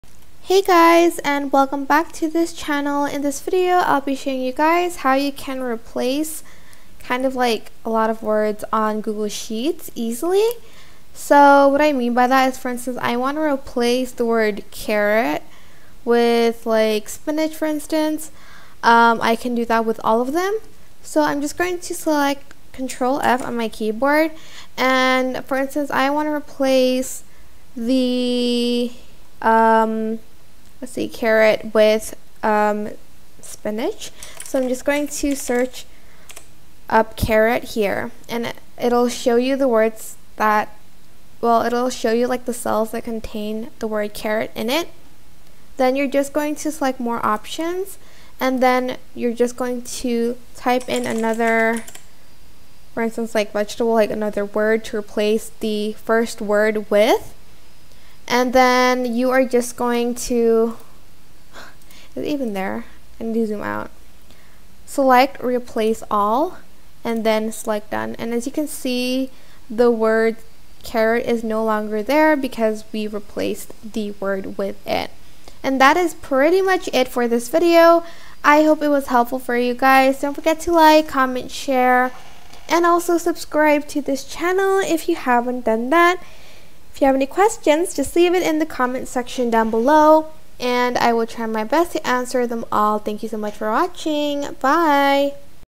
How To Easily Find And Replace Words In Google Sheets
This tutorial will show you how you can easily find and replace words in Google Sheets.